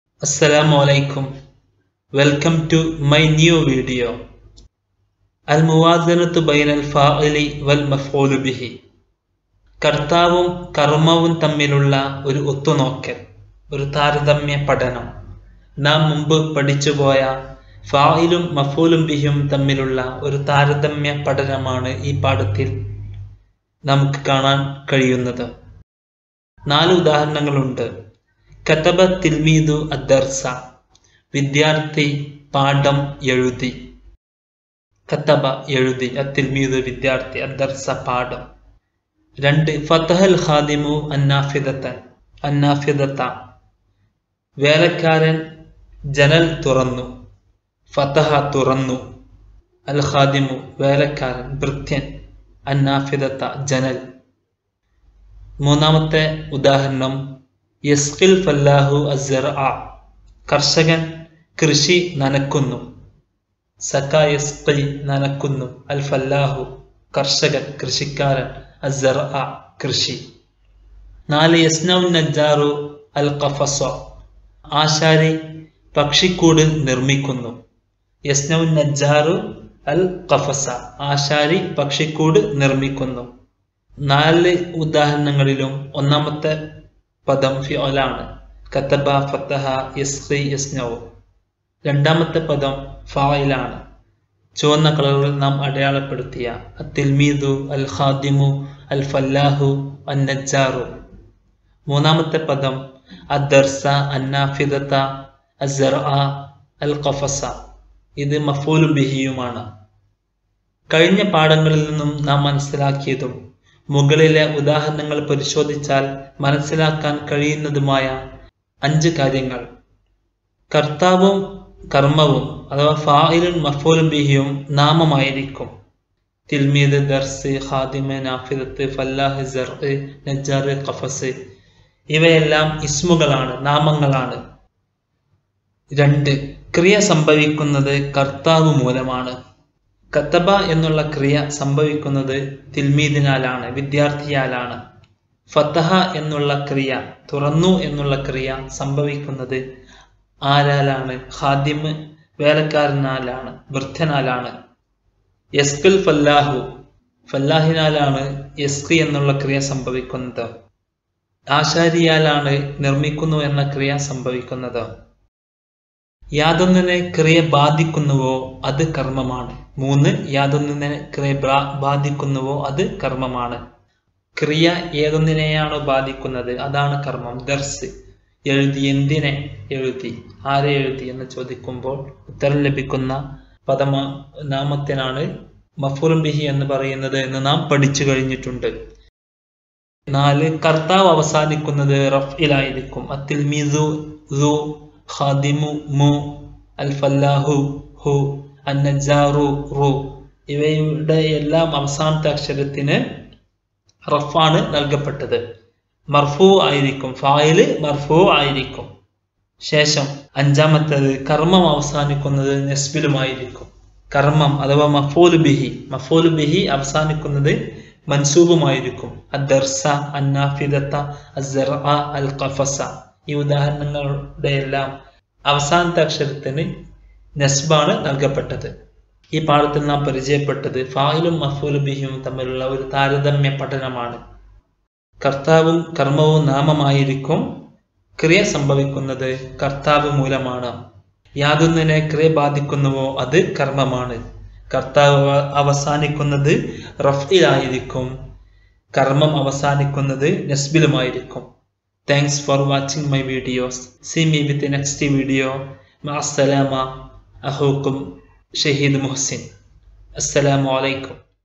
ARABIC GRAMMAR LESSON 8(الموازنة بين الفاعل والمفعول به) IN MALAYALAM
COMPARE FAYIL WITH MAFOOL BIHI